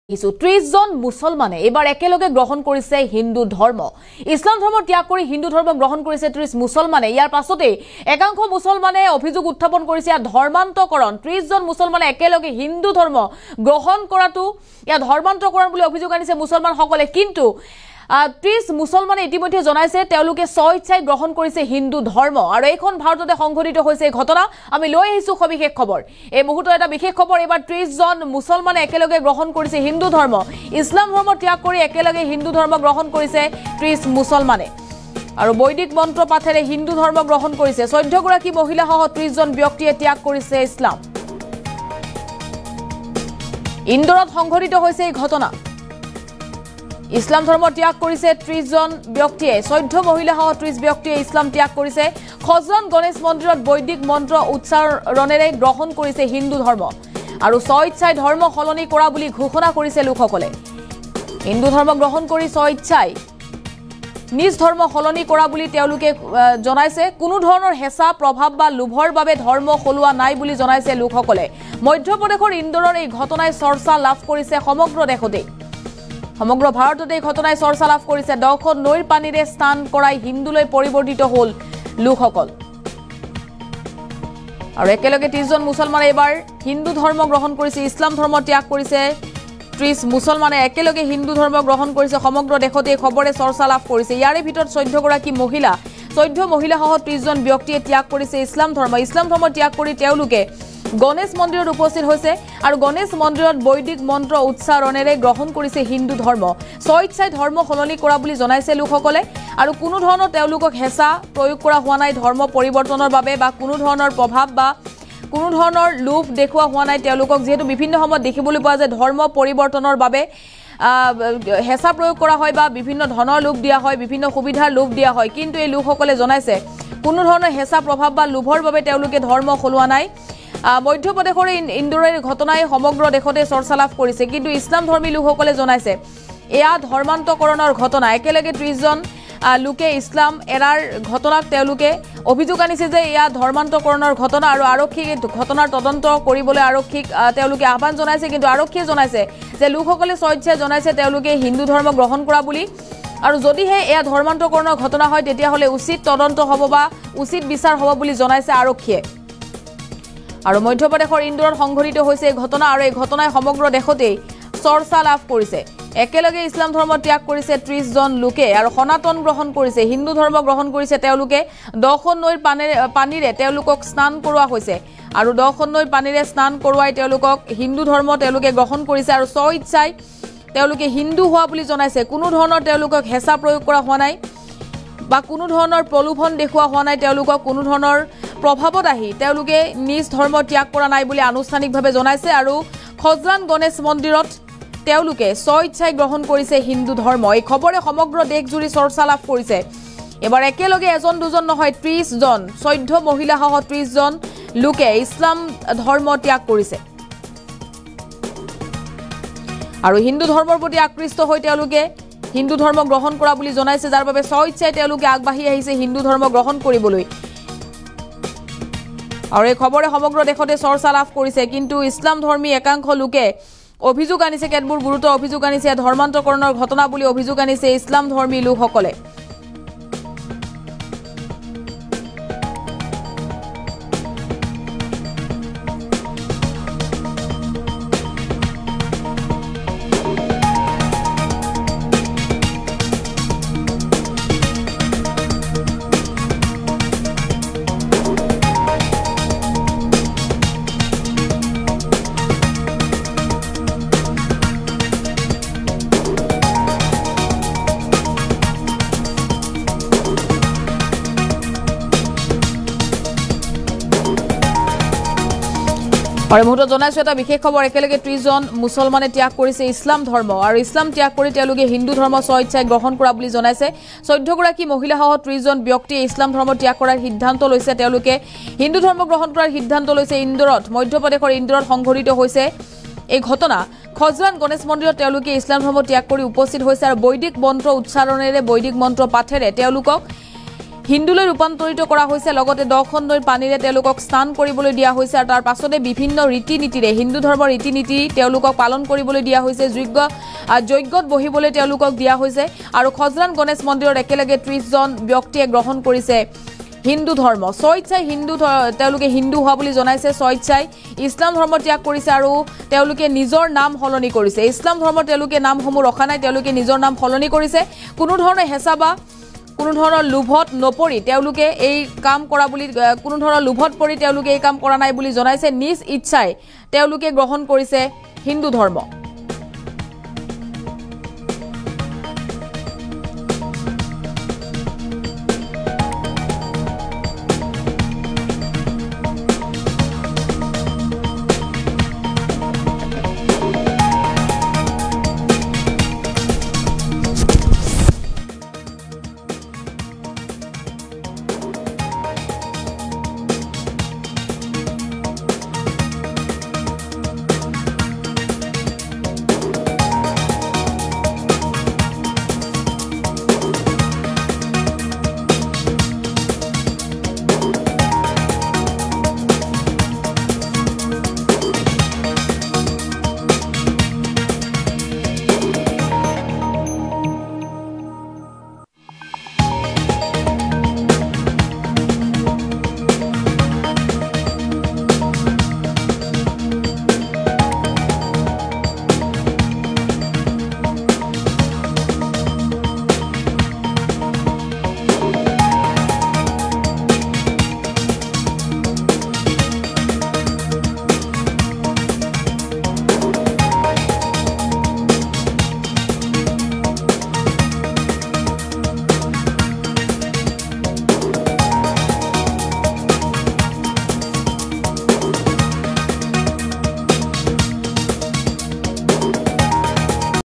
৩০জন সংখ্যালঘুয়ে গ্ৰহণ কৰিলে হিন্দু ধৰ্ম।
৩০জন সংখ্যালঘুয়ে গ্ৰহণ কৰিলে হিন্দু ধৰ্ম। গণেশ মন্দিৰত বৈদিক মন্ত্ৰ উচ্চাৰণেৰে গ্ৰহণ কৰিলে হিন্দু ধৰ্ম। ১০খন নৈৰ পানীৰে স্নান কৰাই হিন্দুলৈ পৰিৱৰ্তিত কৰা হ'ল লোকসকলক।